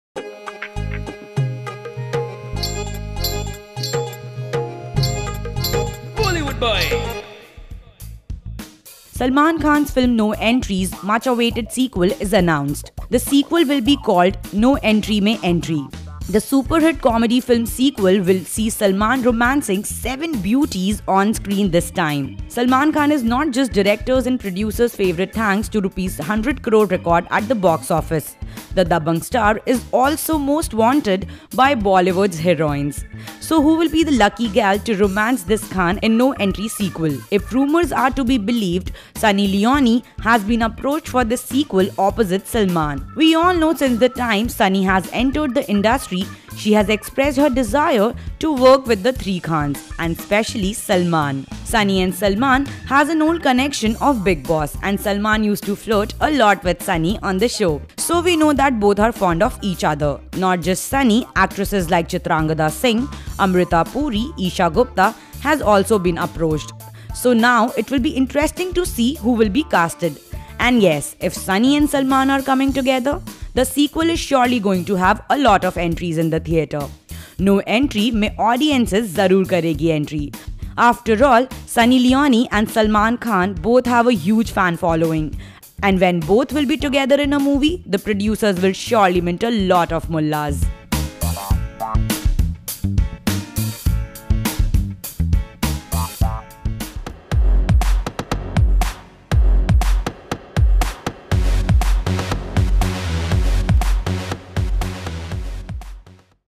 No Entry Mein Entry with Salman Khan And Sunny Leone Hot Romance
No Entry Mein Entry | Salman Khan And Sunny Leone Hot Romance - Salman Khan's film No Entry's much awaited sequel is announced by Boney Kapoor.The sequel will be called No entry mein entry.The superhit comedy film's sequel will see Salman romancing seven beauties on screen this time. Salman Khan is not just directors and producers favourite thanks to his Rs 100 crore record at the box-office. The Dabangg star is also most wanted by Bollywood's heroines. So, who will be the lucky gal to romance this Khan in No Entry sequel?If rumours are to be believed Sunny Leone has been approached for this sequel opposite Salman.We all know since the time Sunny ahs entered the industry she has expressed her desires to work with the 3 khans and specially Salman.